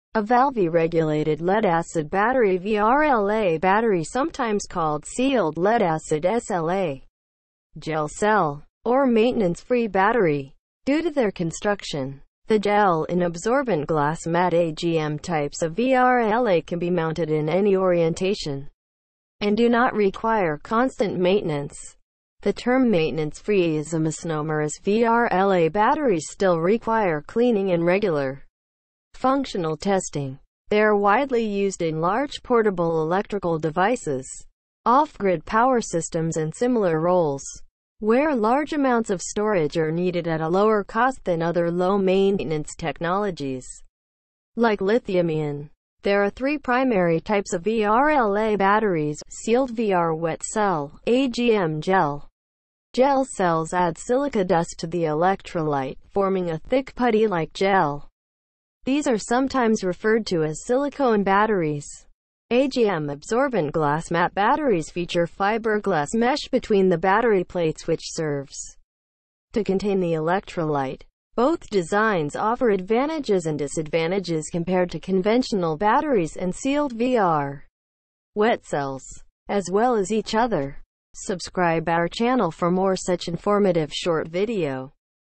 Learn about vrla battery | what is agm battery
A detail information about what is agm battery. I do not own it. I just make video out of this so that some people get some help.